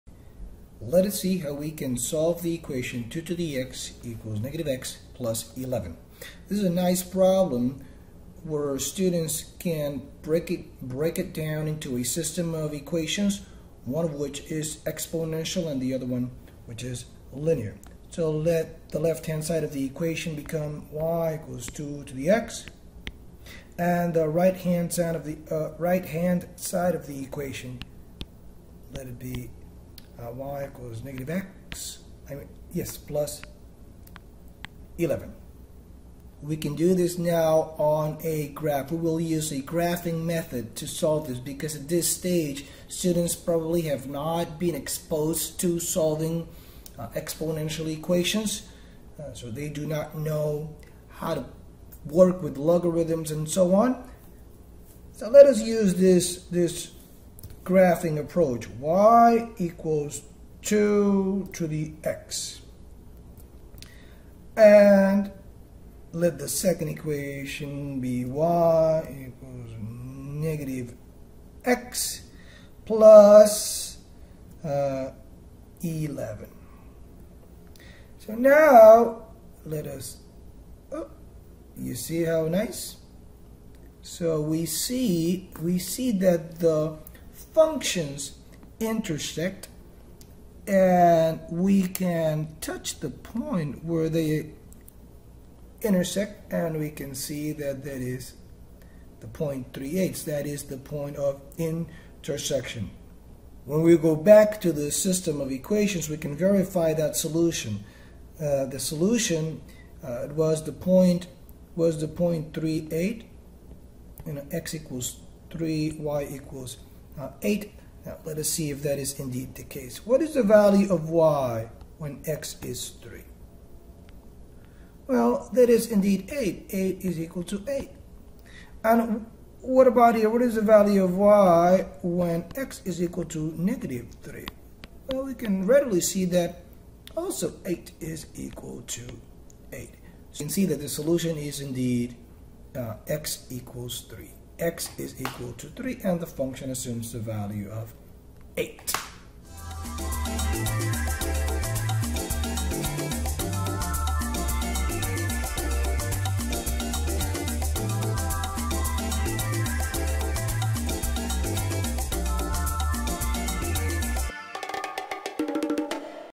Graphing Solution: Applications of Exponential Functions
This lesson is designed to help students apply mathematical models that use basic exponential functions and to show them how to solve basic equations involving exponential functions using graphs. Here we use a graphing approach to solve this equation, which is particularly valuable if students have not encountered logarithms.

Go here for guiding questions, notes for teachers, and example problems